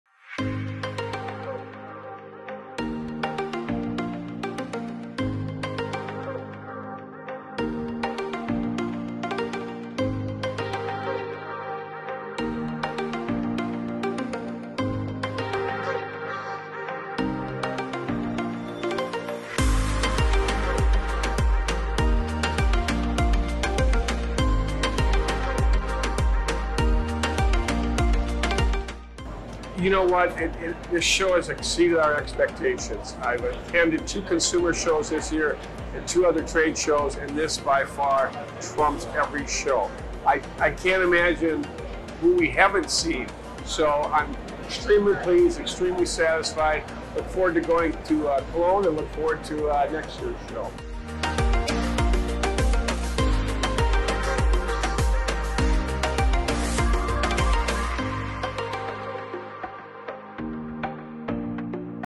h+h americas 2022 - launches successful premiere
First offspring of the world's leading trade fair "h+h cologne”
 Hosting various soft crafts industries under one roof
 195 exhibitors and more than 2,000 attendees
 35 + conference sessions
 Digital After Show – the week after show closing
Many smiles and happy faces were seen during the first edition of h+h americas
which took place from 22-24 June 2022 at the Donald E. Stephens Convention &
Conference Center in Rosemont/Chicago. Right off the bat this trade show and
conference gathered 195 exhibitors and more than 2,000 attendees during 2 ½ days
of activities on the show floor and extensive conference sessions.